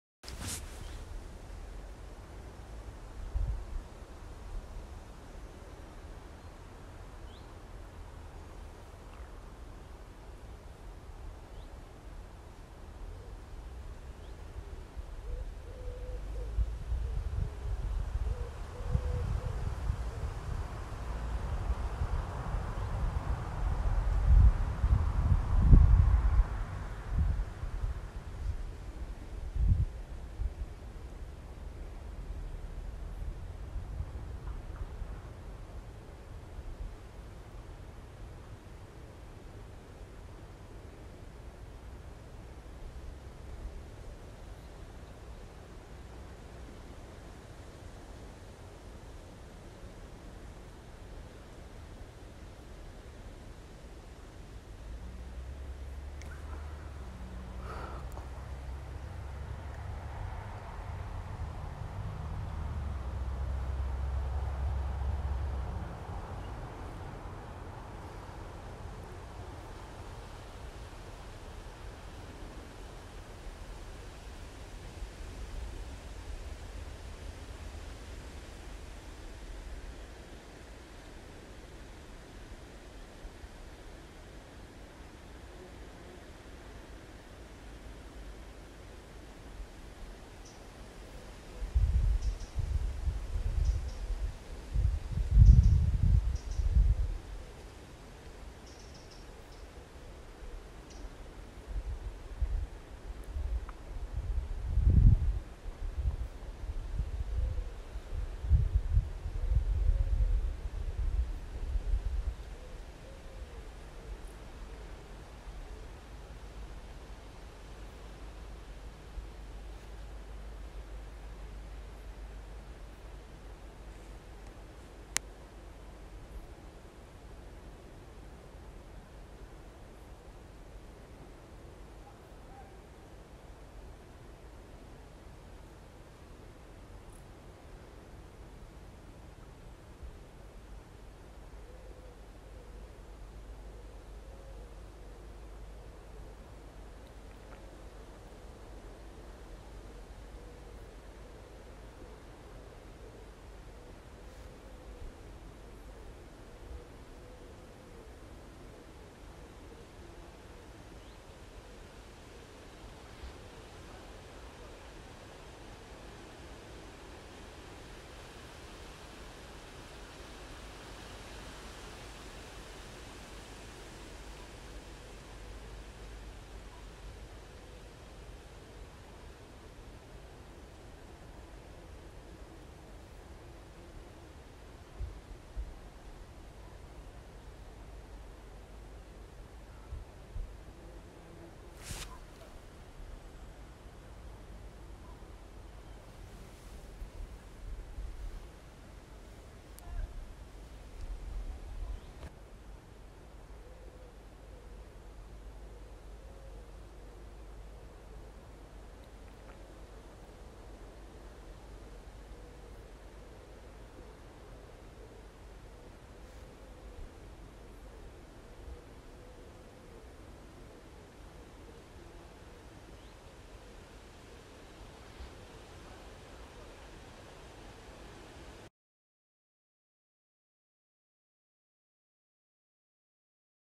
BITTERN AT MINSMERE
Superb view of a stunning Bittern hunting in the reeds at RSPB Minsmere.